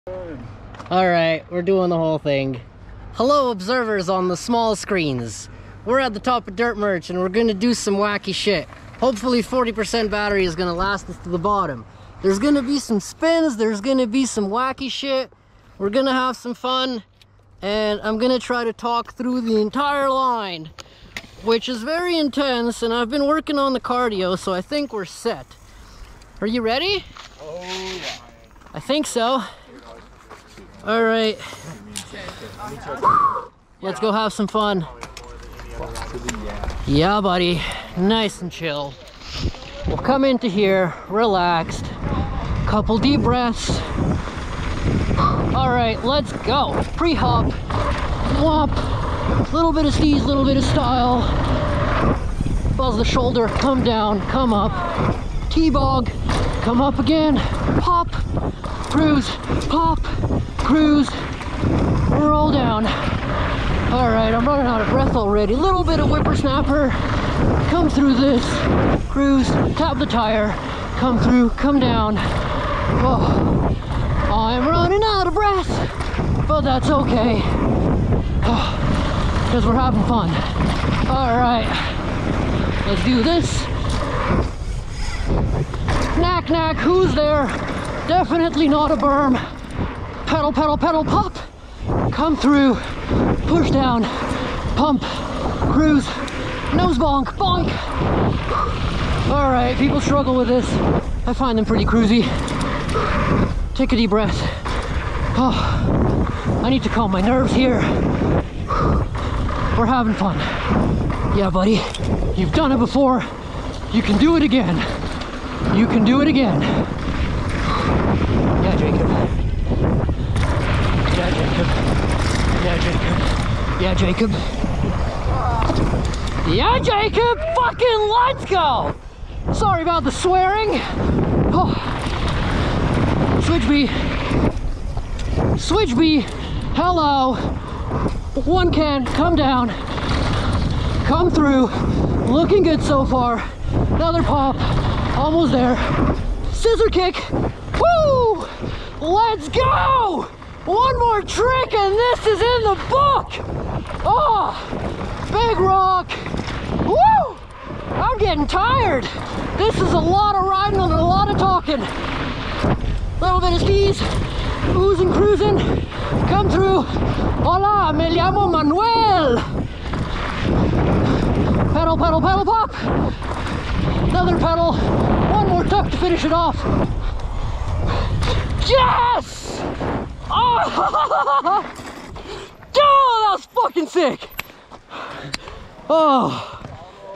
Dirt Merchant Slopestyle lap
Join in for a POV lap down Dirt Merchant in the Whistler Bike Park where I do some tricks, have some fun on the PRO line and don't stop yapping all the way down. Despite it being pretty intense cardio having to talk, ride and focus on doing a couple tricks down the line,  I did my best! :)
Hope you enjoyed the POV,
let me know if you would like to see more of these!